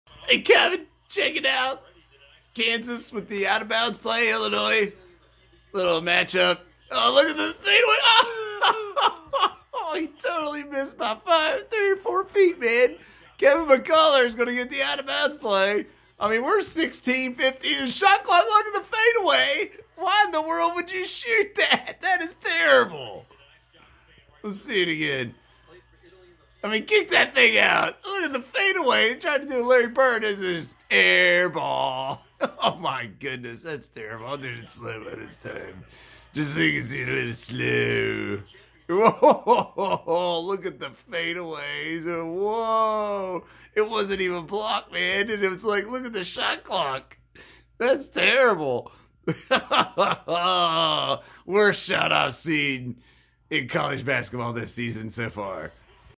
Worst Shot I've Seen in CBB this Year SO FAR - a fadeaway prayer with 15 seconds on shot clock - WOW
Worst Shot I've Seen in CBB this Year SO FAR - a fadeaway prayer with 15 seconds on shot clock - WOW - Kevin McCullar Jr. shoots a fadeaway jumper from an out of bounds play that doesn't even come close with WAY too much time in the shot clock remaining.